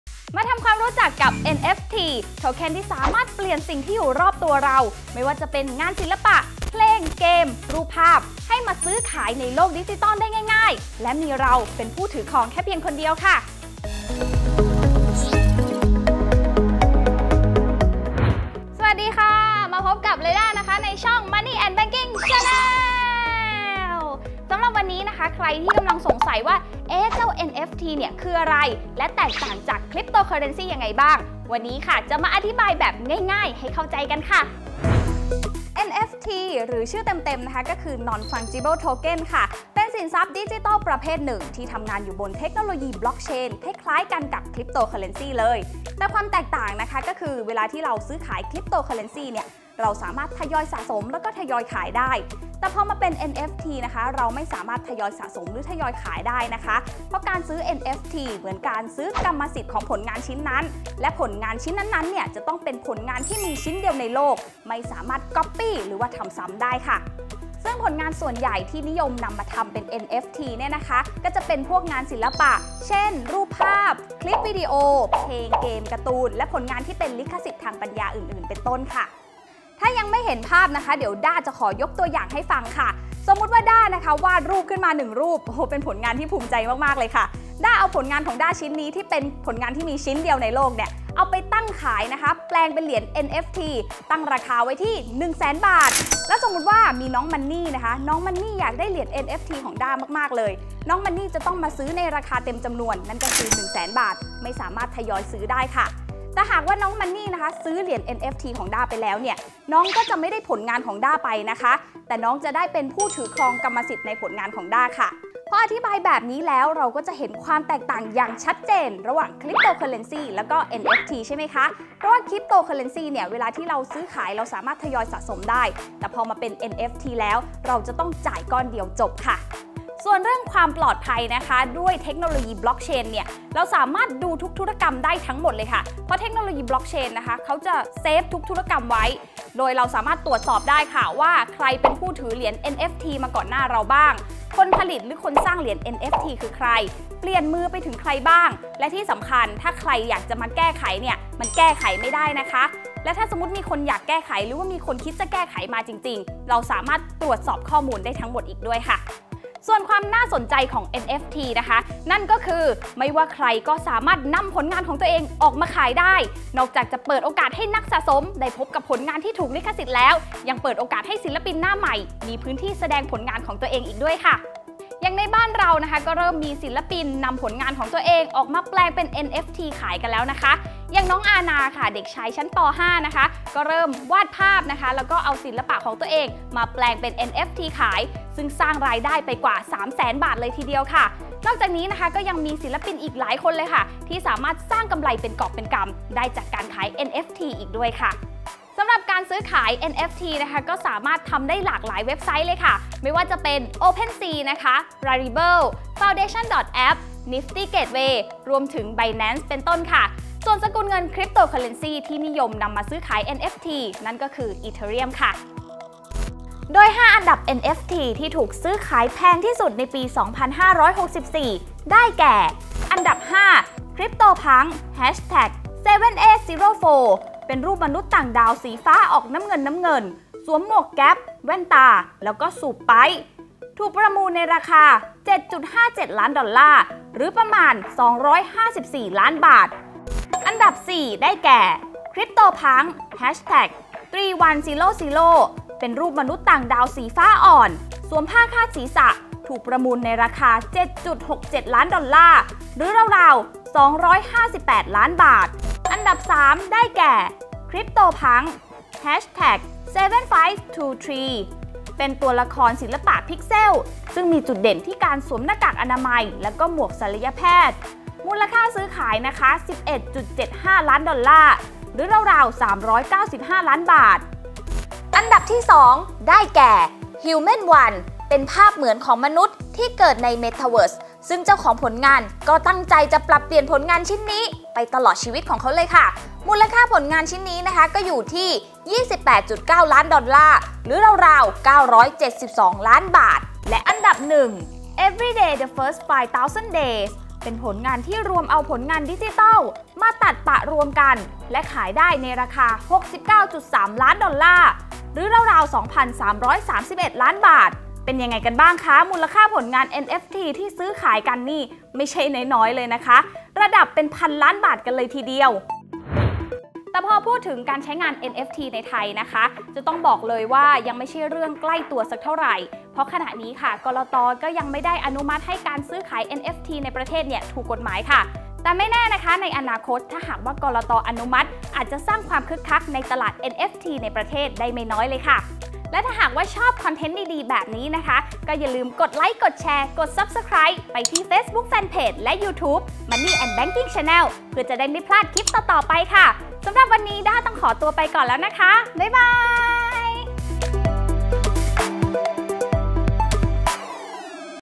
NFT ดิจิทัลอาร์ต สุดต๊าชช!! ที่คนรักงานศิลปะ ห้ามพลาด!!
ทำความรู้จักกับ “NFT” โทเคน ที่สามารถเปลี่ยนสิ่งที่อยู่รอบตัวเรา ทั้งงานศิลปะ 🖼 เพลง 🎼 เกม 🎮 และผลงานอื่นๆ อีกมากมาย ให้สามารถซื้อขายบน “โลกดิจิทัล” ได้ง่ายๆ และเราเป็นผู้ถือครองเพียงคนเดียว

⭐️ NFT หรือชื่อเต็มๆ คือ Non-Fungible Token  เป็นสินทรัพย์ดิจิทัล ที่อยู่ใน “Blockchain” คล้ายๆ กับ “Cryptocurrency” แต่จะแตกต่างกันตรงที่ คริปโตฯ เวลาซื้อขายเราสามารถทยอยสะสมและทยอยขายได้ แต่ NFT จะเหมือนกับการซื้อขาย “กรรมสิทธิ์ของผลงาน” และจะต้องเป็นผลงานที่มีชิ้นเดียวในโลก ไม่สามารถทำซ้ำหรือคัดลอกได้ ✅

👉 ดูคลิปนี้จบ ชอบไม่ชอบ เลิฟไม่เลิฟ กับตัว NFT คอมเม้นบอกต่อกันได้นะคะ ส่วนสายเกมอย่างแอดแอด แอบแอบเล็งอยู่นะจ้ะ 👀